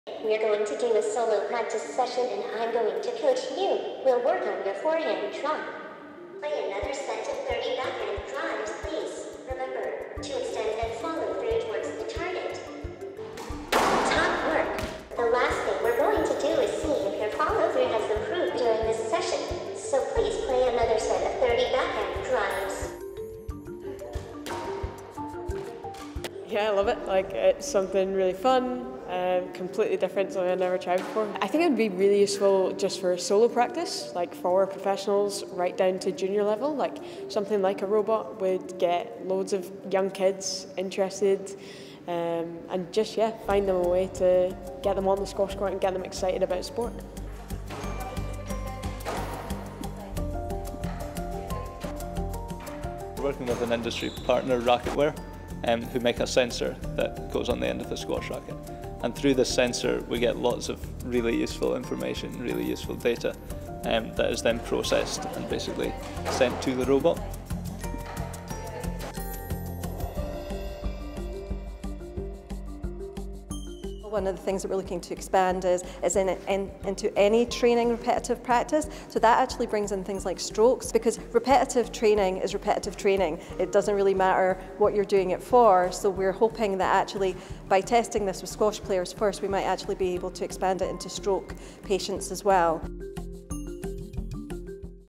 Meet Pepper, the world’s first robot squash coach
t’s one of the most physically demanding sports going, so many amateur squash players must wish they had the stamina of a robot.

While a new invention won’t make that possible, they can at least now learn their strokes from a coach who never makes a mistake.

Pepper, the world’s first android squash trainer, has taken up residence at Heriot-Watt University in a joint project with Edinburgh University.

The multi-million pound addition to the National Robotarium has gone down well with professional players, who believe it could transform the sport.